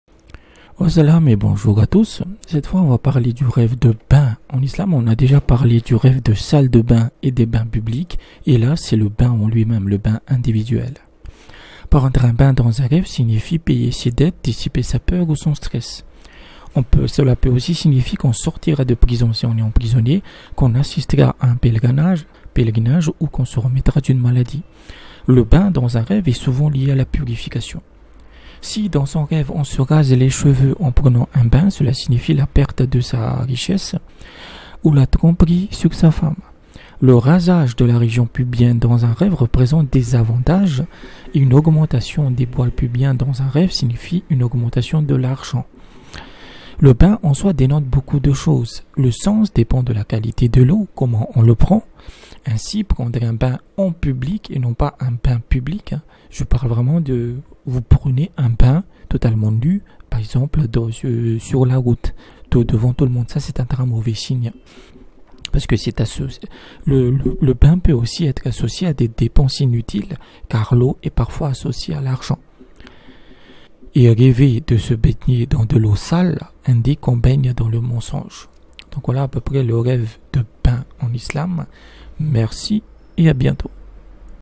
Rêver de bain en Islam
L'interprétation des rêves en Islam sur le bain.

La plupart des photos proviennent de sites comme Pixabay ou Unsplash.